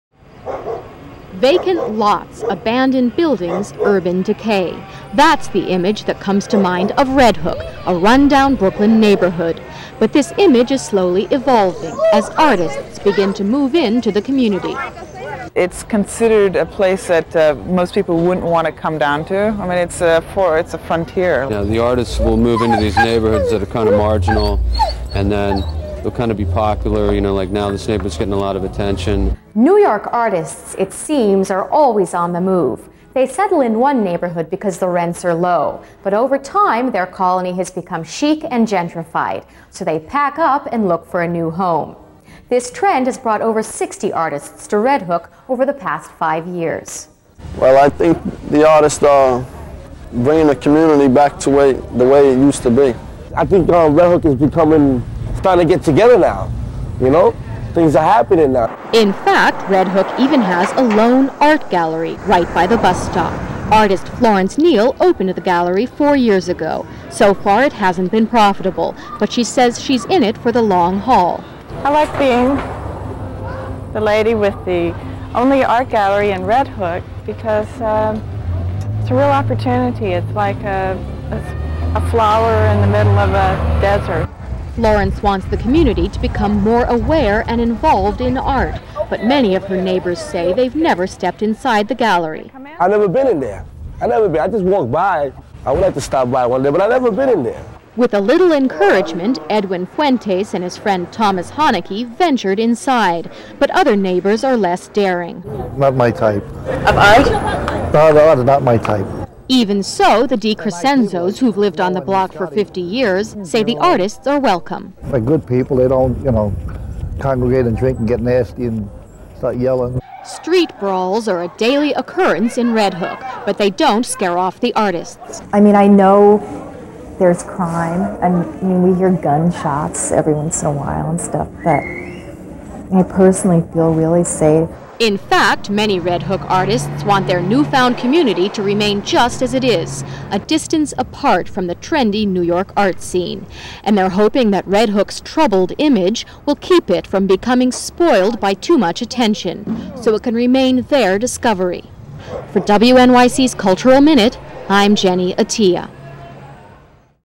Red Hook, Brooklyn, before the Gentrification
On a beautiful spring day in the mid 1990s, I meander the streets of Red Hook, when it's still a rundown Brooklyn neighborhood. I meet its first art gallery owner, and the two longshoremen who venture inside. This is one of my favorite stories I had a chance to do for WNYC TV, the PBS station I worked for in Manhattan. (This station too is now history.)  Let me know what you think!